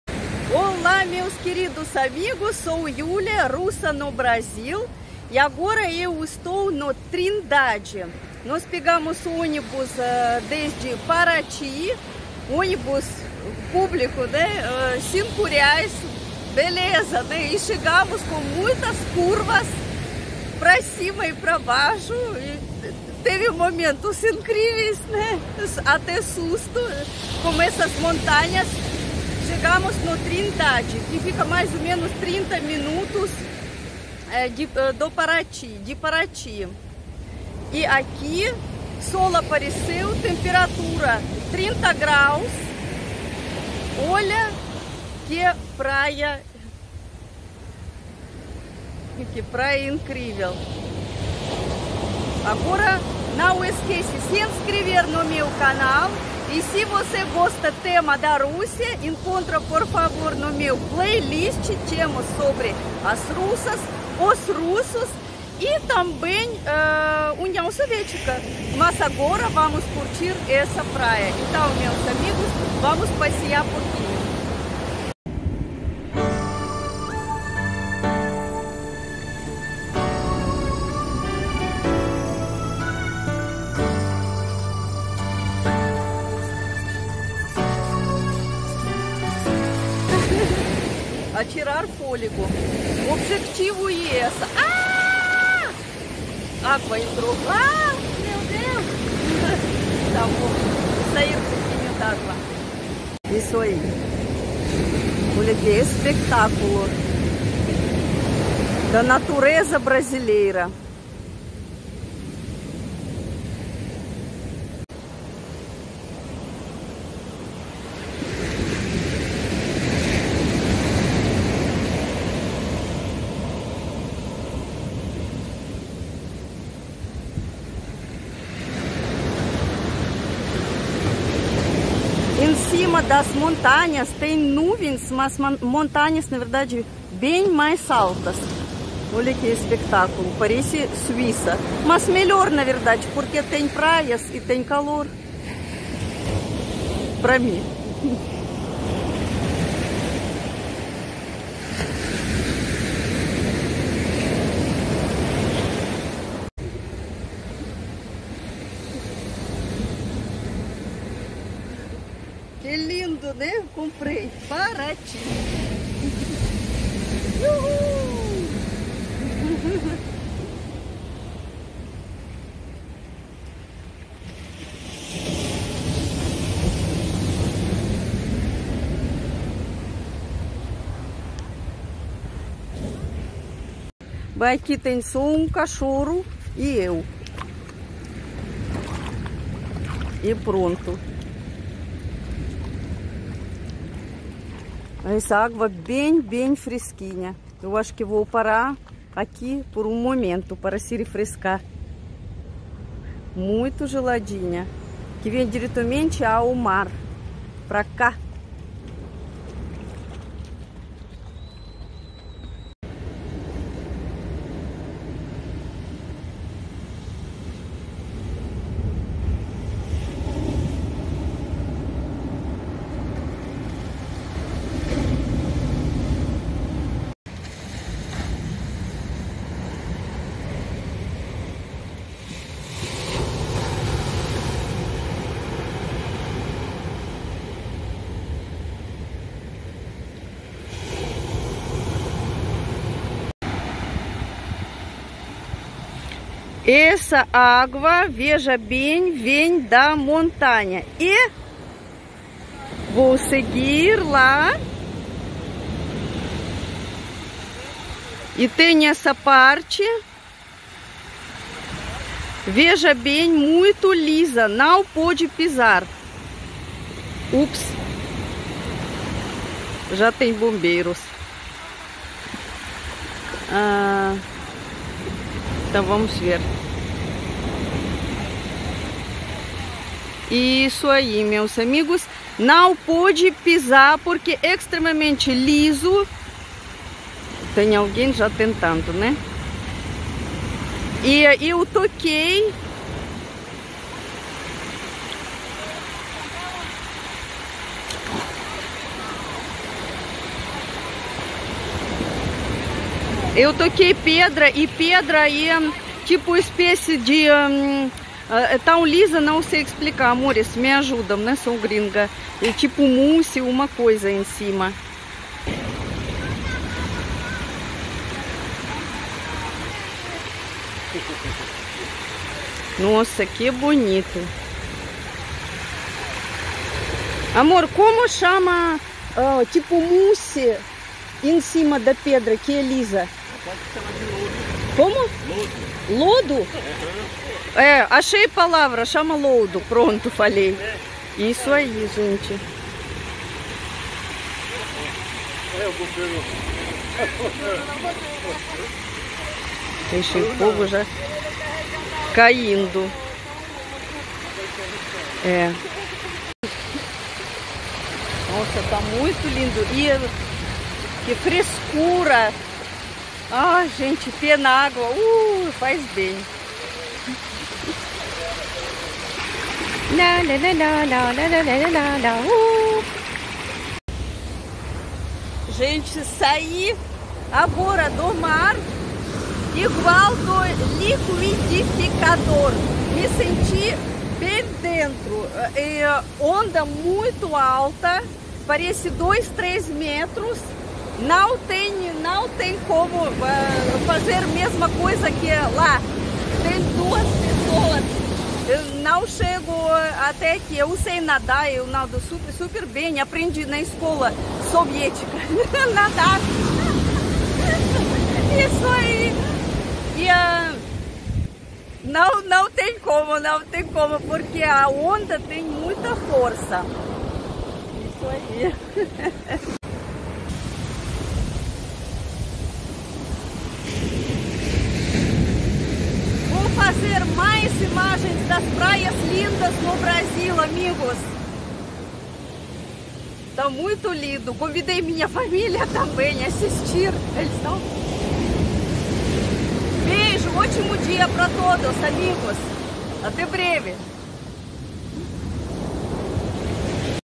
GRINGA SIBERIANA🙆❄️ CURTA MELHORES Praias do Brasil, TRINDADE RJ.
GRINGA SIBERIANA  NAS MELHORES praias do Brasil,  TRINDADE RJ.
As praias mais bonitas,  desertas ou vazias.

Agradeço todos apoiadores do meu canal !

AGRADEÇO qualquer ajuda ajuda!🙏